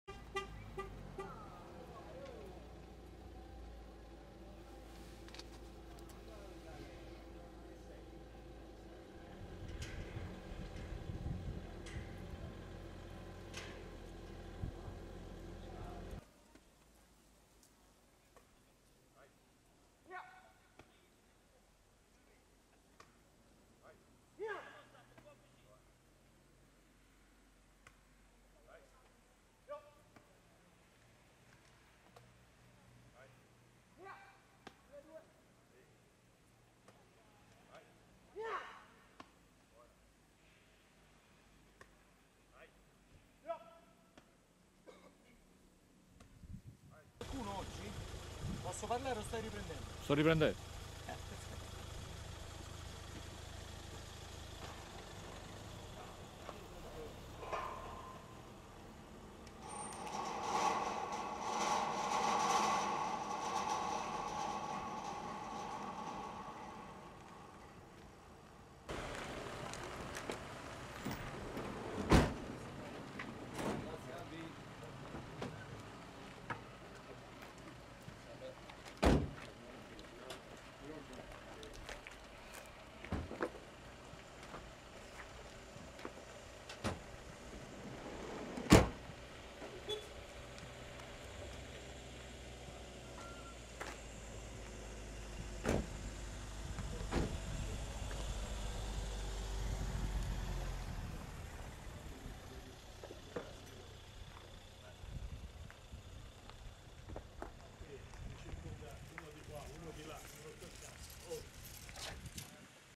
Avellino, terzo giorno di ritiro a Sturno: in prova il francese Kerbache
Braglia aspetta Bruzzo e Miceli e testa l'esterno offensivo transalpino, classe 2000.